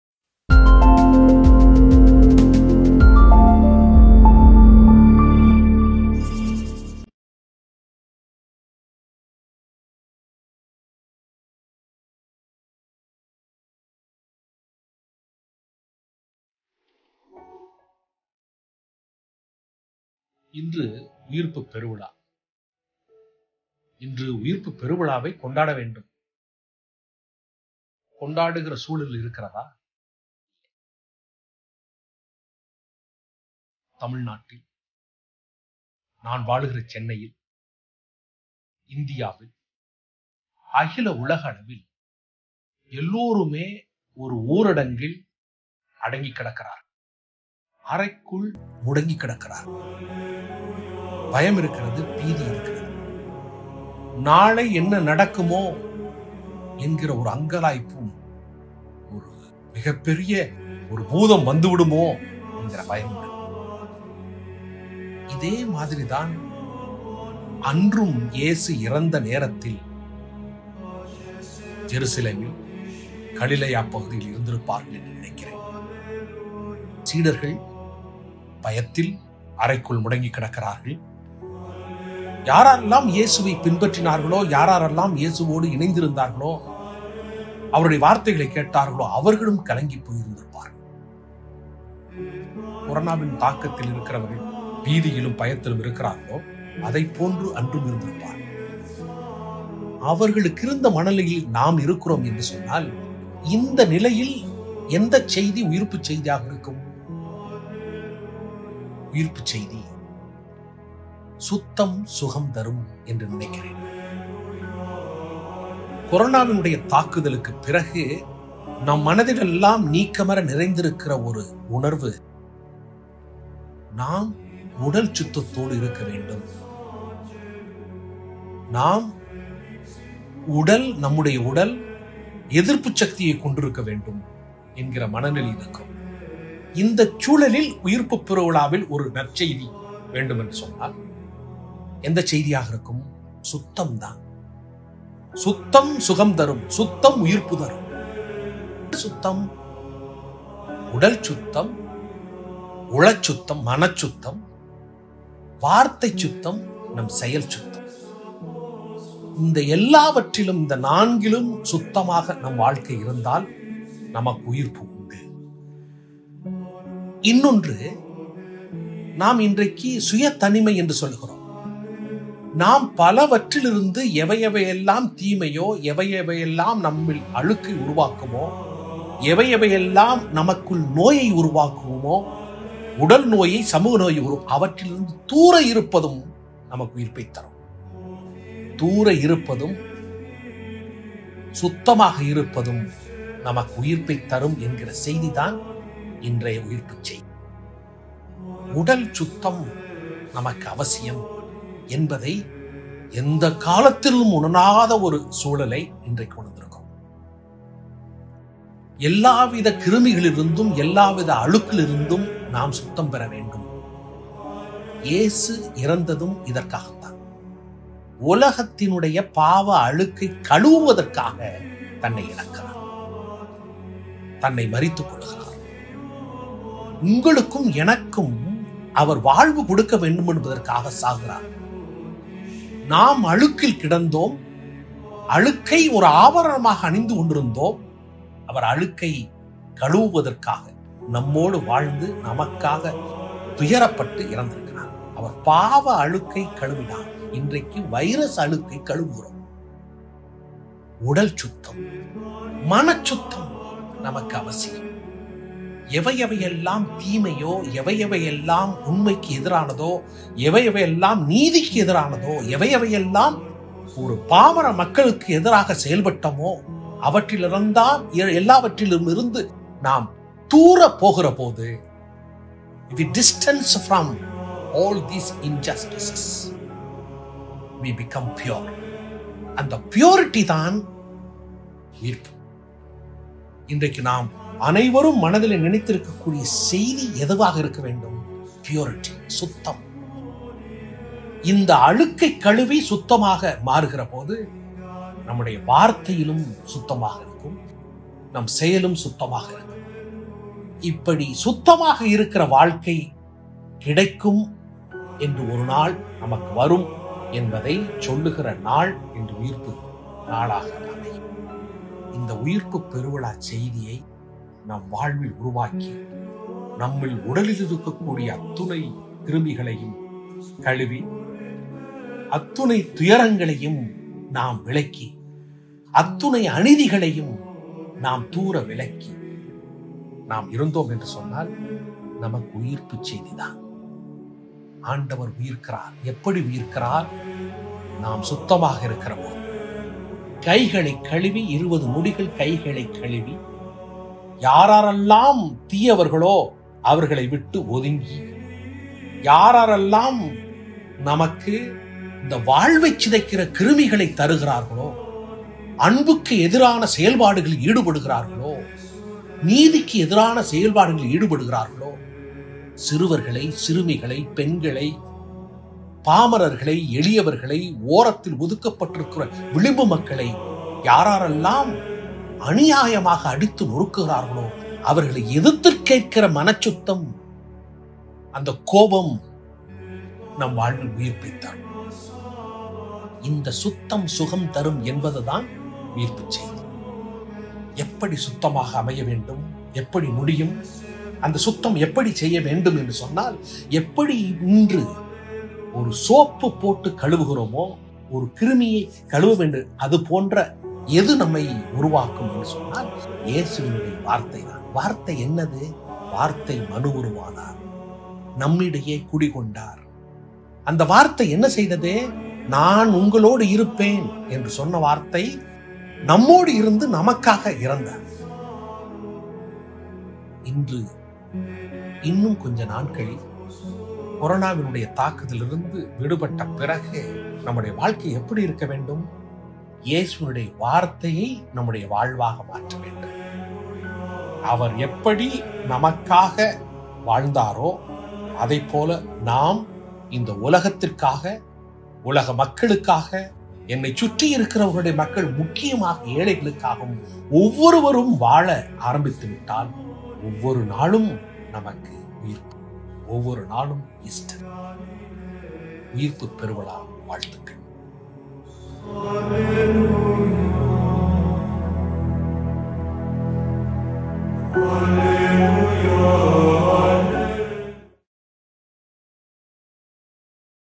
சுத்தம் தரும் சுகமே உயிர்ப்பு | Easter Message by Rev. Fr. Joe Arun SJ
சுத்தம் தரும் சுகமே உயிர்ப்பு - உயிர்ப்புப் பெருவிழா

இன்று உயிர்ப்பு பெருவிழாவைக் கொண்டாடவேண்டும். கொண்டாடும் சூழ்நிலையில் இந்த உலகமும், நம் நாடும் இருக்கின்றதா. இல்லை. எல்லாருமே உள்ளே முடிங்கிக் கிடக்கிறார்கள். நாளை என்ன நடக்குமோ என்ற அங்கலாய்ப்பும் பயமும் இருக்கிறது. இதேபோலதான் இயேசு இறந்தபோது இயேசுவைப் பின்பற்றியவர்களும் அவர் வார்த்தையைக் கேட்டவர்களும் இருந்திருப்பார்கள்.  அவர்கள் மனநிலையில்தான் நாமும் இன்று இருக்கிறோம்.

இந்த சூழ்நிலையில் உயிர்ப்பு பெருவிழாவின் செய்தி எதுவாக இருக்கும். சுத்தம் சுகம் தரும். இன்று கொரோனா தாக்காமல் இருக்க நாம் உடல் சுத்ததோடு இருக்கவேண்டும். நம்முடைய உடல் எதிர்ப்புசக்தி கொண்டிருக்கவேண்டும் என்று சொல்கிறார்கள்.

சுத்தம் என்பது உடல், உளம், மனம், செயல் சுத்தம். சுயத்தனிமை, எவையெல்லாம் நம்மில் நோயை உண்டாக்குமோ அதிலிருந்து நாம் விலகி இருப்பதுதான் உயிர்ப்பினைத் தரும். எல்லாவிதமான அழுக்கிலிருந்து நம்மைக் கழுவிக்கொள்ளவேண்டும்.

இயேசு இறந்ததும் இதற்காகத்தான். உலகத்தின் அழுக்கை கழுவுவதற்கு தம்மையேக் கொடுத்தார். நாம் அனைவரும் அழுக்கை ஆபரணமாக அணிந்துகொண்டோம். எனவே நம்மை தூய்மைப்படுத்த நமக்காக துயரப்பட்டு இறந்தார். இன்று வைரஸ் அழுக்கைக் கழுவ உடல்சுத்தமும். பல்வேறு நிலையில் நடைபெறும் அநீதிகளைக் களைய மனசுத்தம் வேண்டும். தீமைசெய்வதிலிருந்து அநீதி விளைவிப்பதிலிருந்து நாம் விலகிக்கொண்டிருந்தால் நாம் தூய்மையடைவோம், அதுதான் உயிர்ப்பு.

இவ்வுலகில் அன்புக்கு எதிரான செயல்பாடுகளில் ஈடுபடுபவர்களிடமிருந்து விலகி இருப்பதும், அவர்களை எதிர்த்துக் கேட்கும் கோபமும் சுத்தத்தின் வெளிப்பாடு. எனவே உடலை மட்டும் சுத்தம் செய்வதோடு நின்றுவிடாமல், நம்முடைய உள்ளத்தை தூய்மைப்படுத்துவோம். உயிர்த்த இறைவன், வார்த்தையான இறைவன் நம்மிலிருந்து செயலாற்றுவார்.

தொடர்பில் கொடுக்கப்பட்ட காணொளிக்காட்சியின் சுருக்கம்